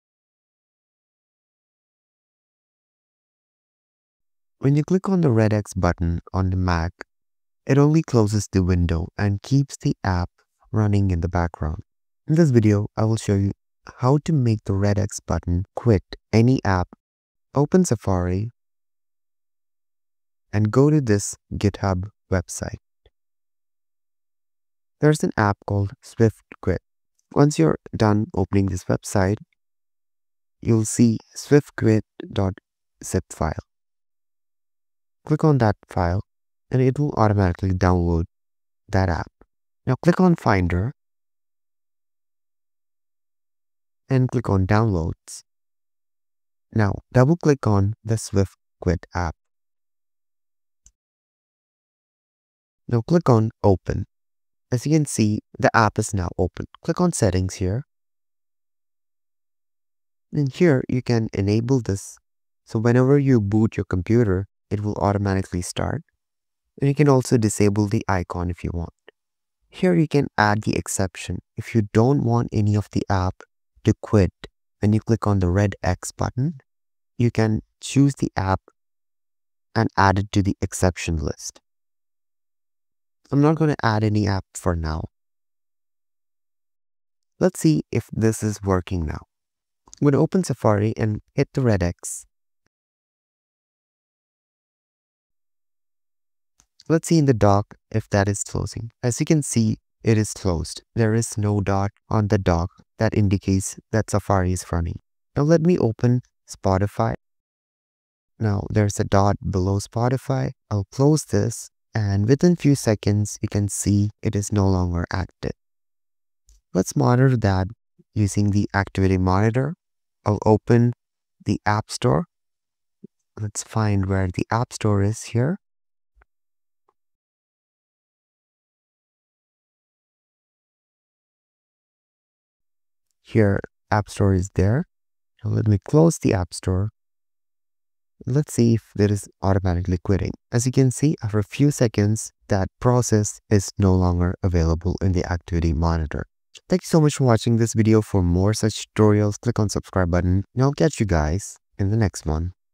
How To Make Red X Button Quit App In macOS Like Windows 11
How to completely close an app by clicking on red button in mac OS.
Tutorial to make mac automatically quit apps when closing their windows (clicking the red x)